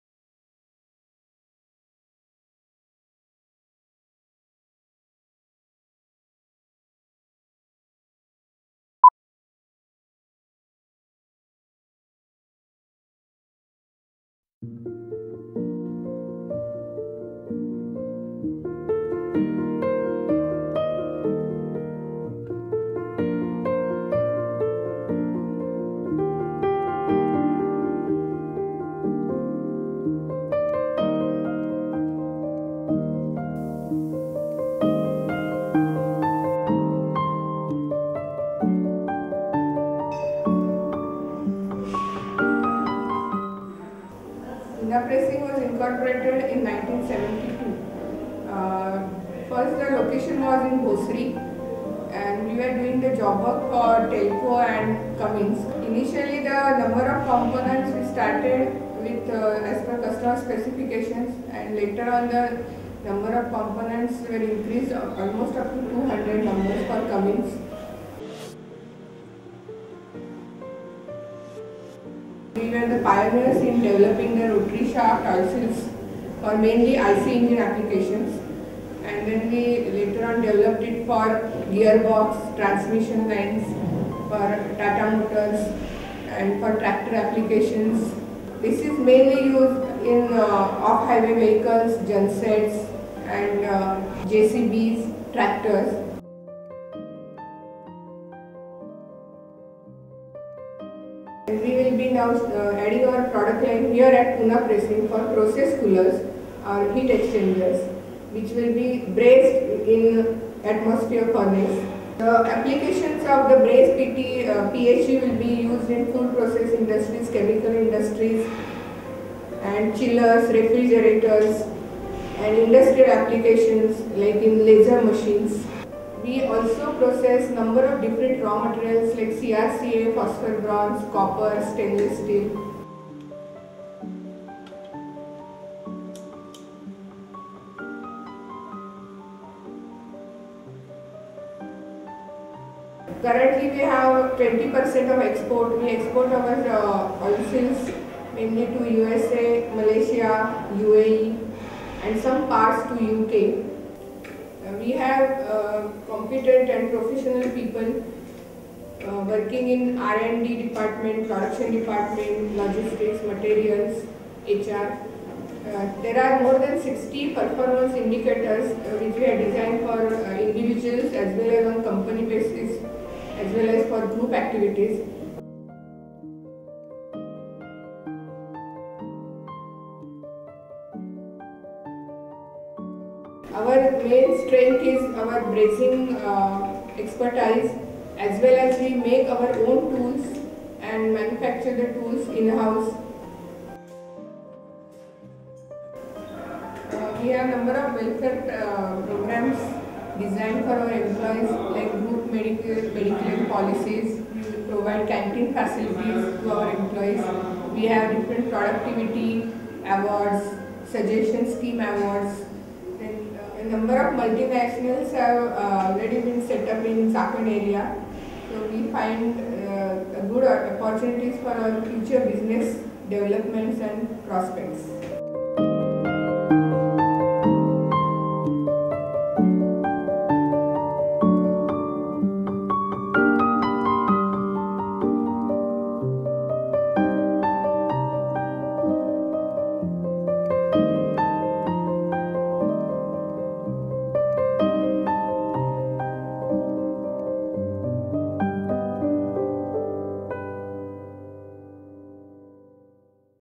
Poona Pressings Ltd. (Infomercial)
Poona Pressings Ltd. is a Pune-Chaakan based firm manufacturing PTFE seals, an integral component of an IC engine. PPPL's success is based on the expertise of its own precision tool making division, rigorous research, unique innovation and an experience of over 25 years.
PPPL supplies PTFE Oil Seals for Engines, Transmission and Gear Box applications. PPPL is a ISO / TS 16949 certified company.

Shot by Pranav Karnad, edited by Venkatesh Harinathan